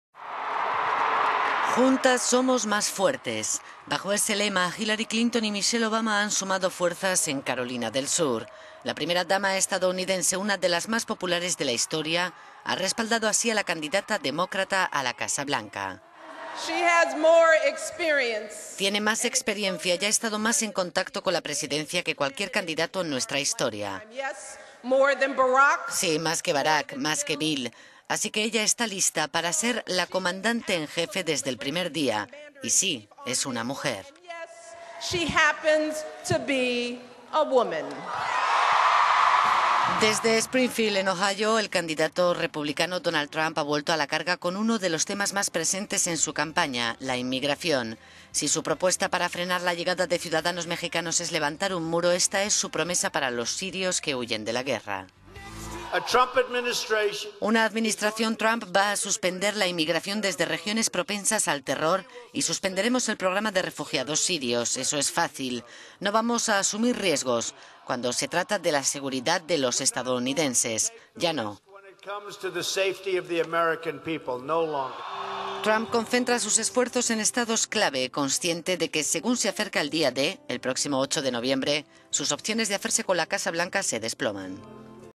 Clinton y Trump echan el resto en la recta final de la campaña - world
"Juntas somos más fuertes". Bajo este lema, Hillary Clinton y Michelle Obama han sumado fuerzas en Carolina del Sur.

La primera dama estadounidense, una de las más populares de la historia, ha respaldado así a la candidata demócrata a la Casa Blanca.

"Tiene más experiencia y ha estado más en contacto con la presidencia que cualquier candidato en nuestra historia. Sí, más que Barack, más que Bill. Así que ella está lista para ser la comandante en jefe desde el primer día y sí, es una mujer", …